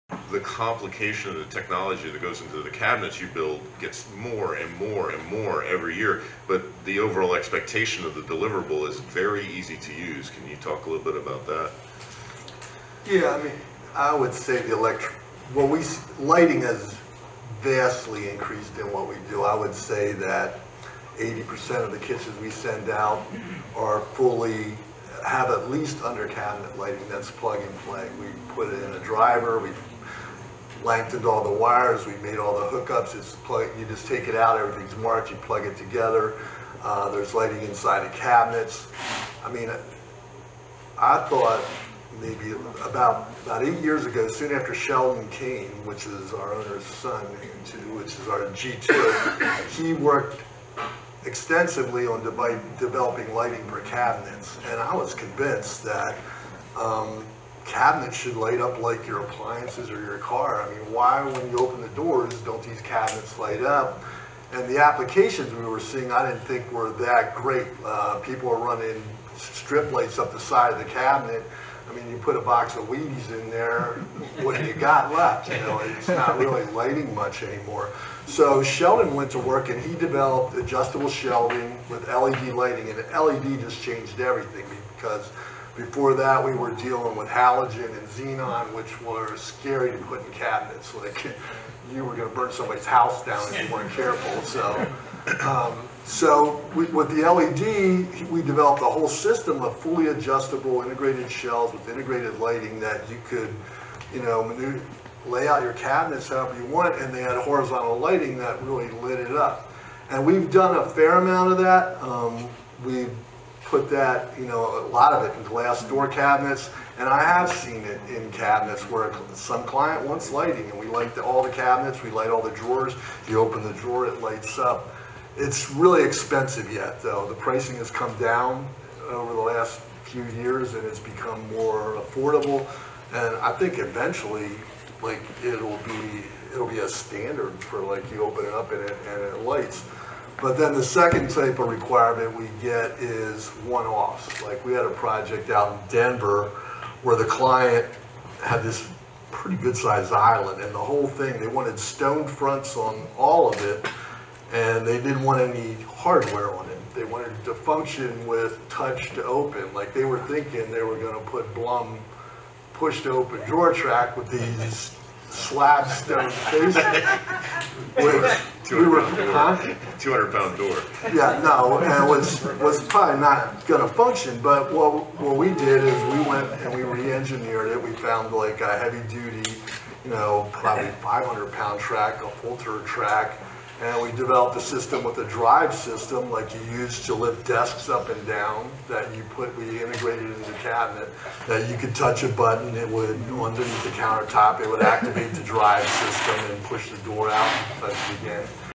NKBA Luxury Design Summit | Rob Martin QA: On easy to use high tech cabinets
A panel discussion led by Paul Grothouse on current trends, business successes, and best practices as they apply to luxury architecture, design, and manufacturing.

Cultivating Successful Partnerships with Architects, Interior Designers and Product Manufacturers.